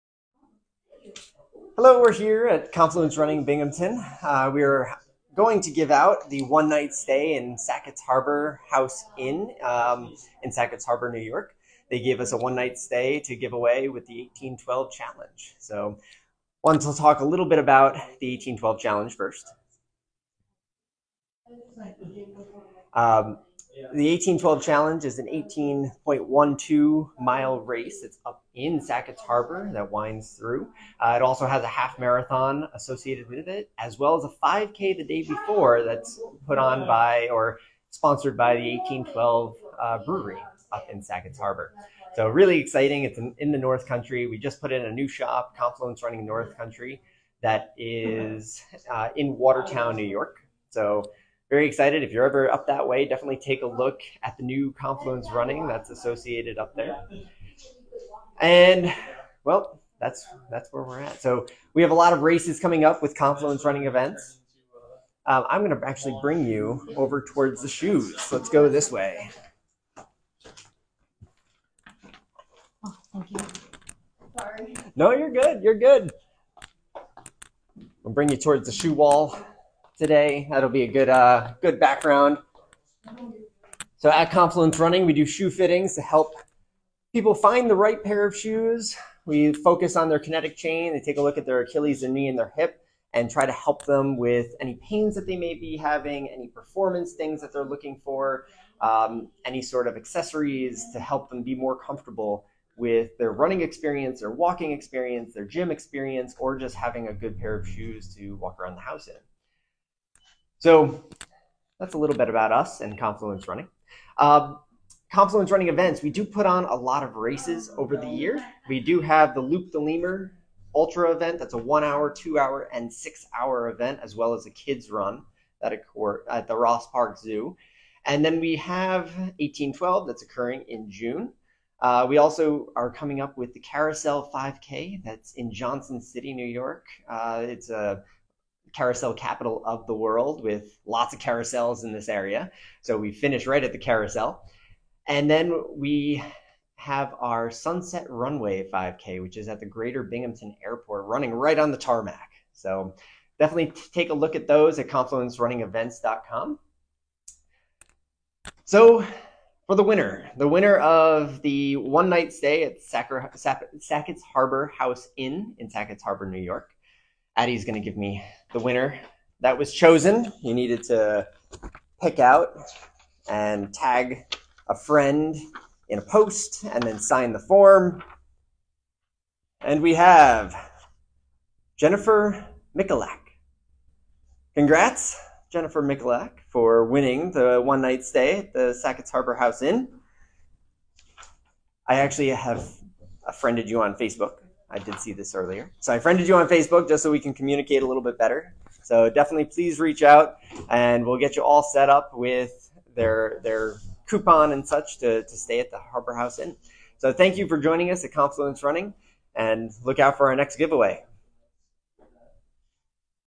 1-Night Giveaway for Sackets Harbor House Inn from the 1812 Challenge and Half Marathon
Explore our latest blog post, which announces the fortunate winner of the 1812 Challenge Giveaway, Jennifer Duhoski Michalak, who has earned a delightful stay at the Harbor House Inn in picturesque Sackets Harbor, NY. This post offers an intimate glimpse into the historic and serene village of Sackets Harbor, highlighting its beautiful landscapes, cultural heritage, and the inviting Harbor House Inn. Readers will find themselves transported to a tranquil getaway, perfect for those looking to unwind by the soothing waters of Lake Ontario while enveloped in the charm of a historic community.